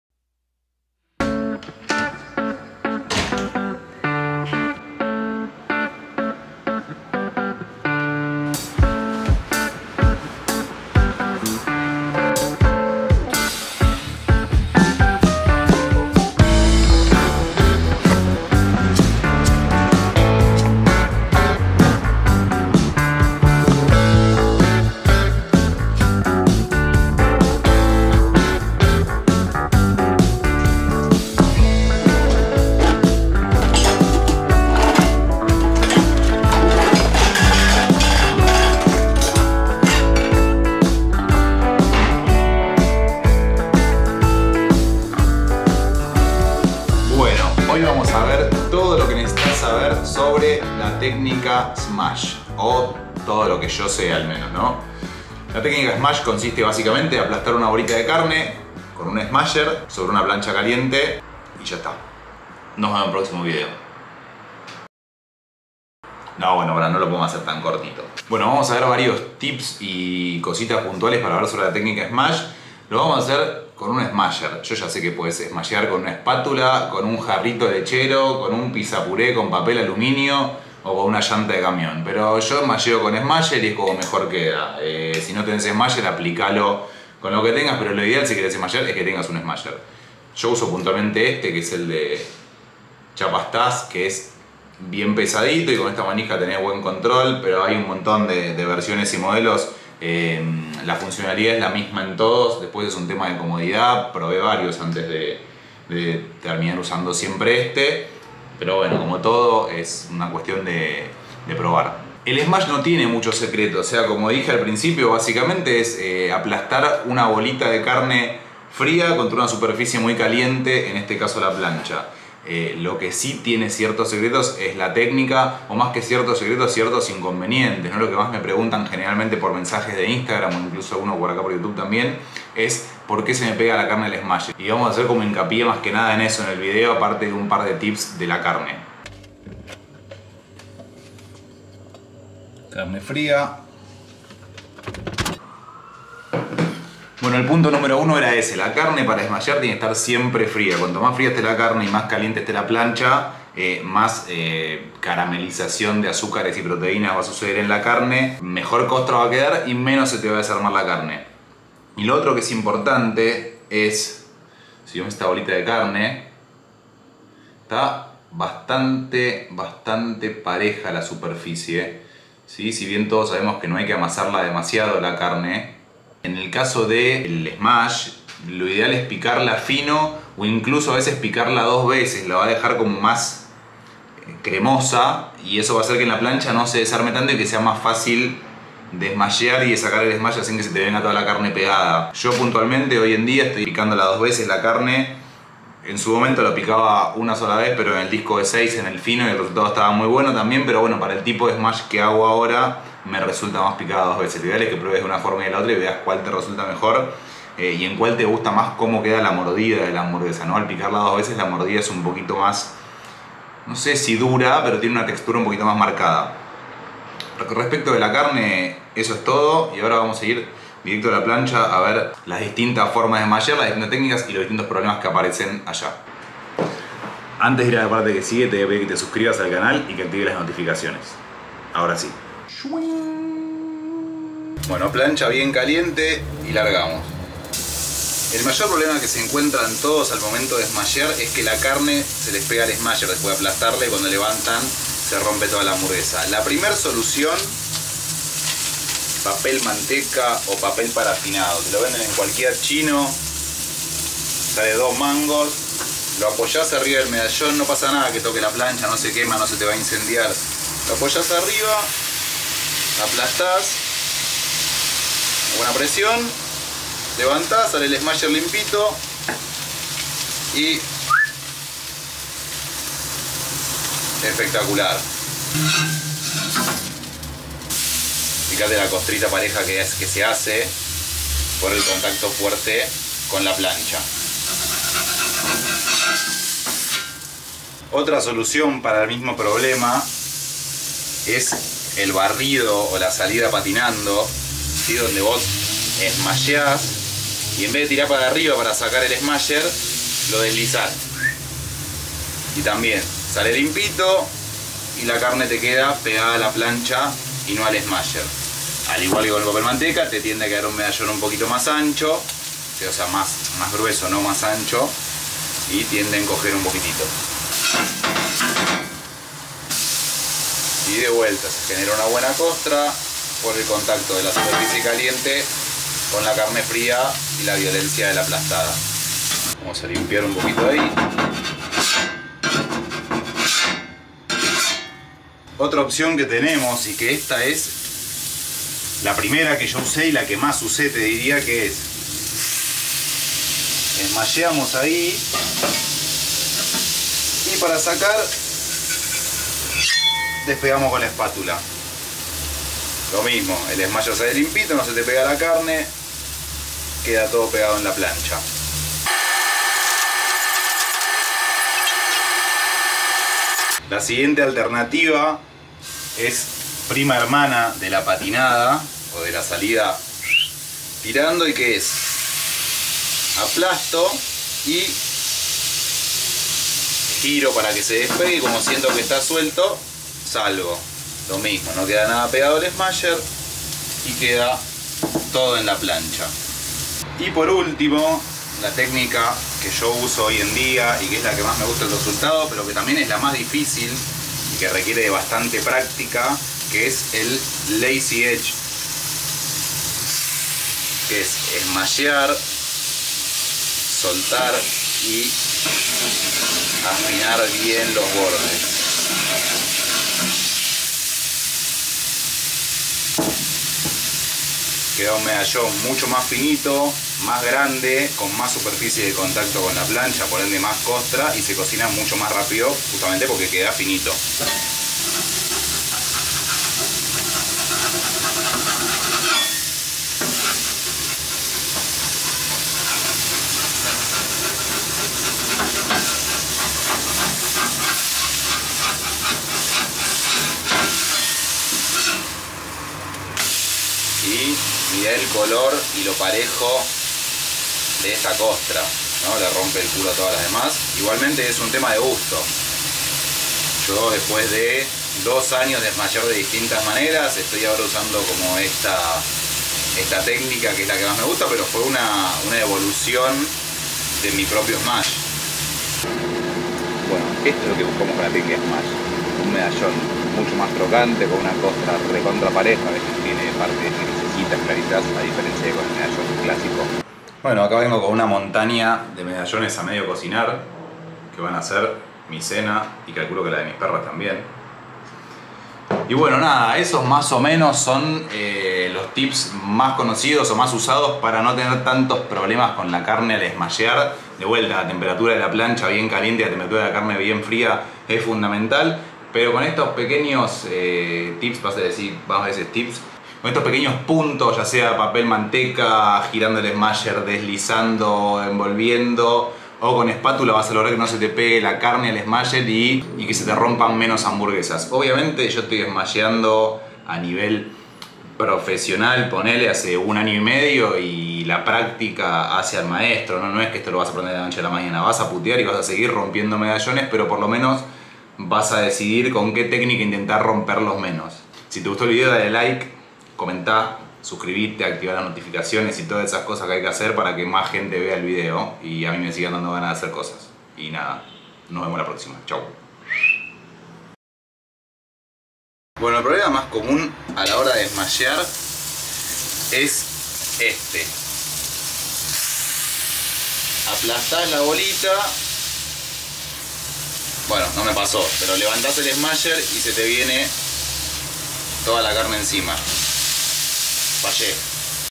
7 SECRETOS para mejorar tus hamburguesas SMASH (o Smashed Burgers) | JohnJohnTips
Después de smashear mas de 60,000 bolitas de carne en los últimos dos años, te enseño algunos tips que me hubiese encantado tener cuando arranque. En cuanto los pongas en practica vas a ver como mejoran tus hamburguesas!

@JohnJohnBurger en Instagram!